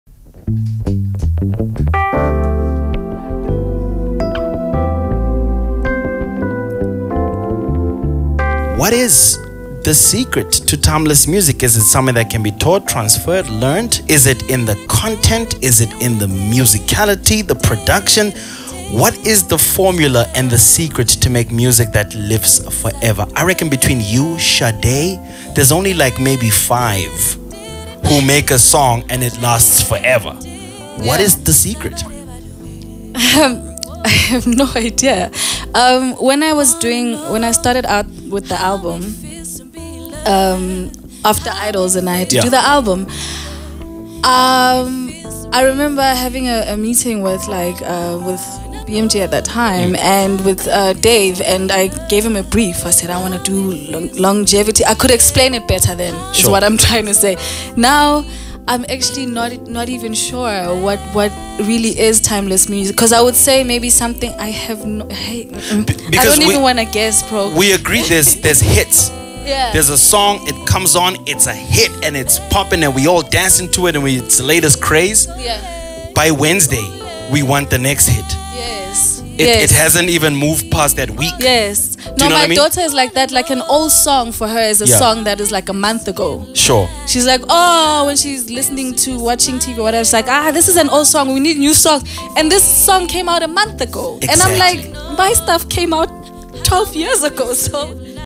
Zamajobe on Kaya TV
With more than a decade in the industry, Zamajobe is still intent on carving her own path musically. While many categorise her as just a soul/jazz artist, she has explored many genres like reggae, funk, dance and afro pop.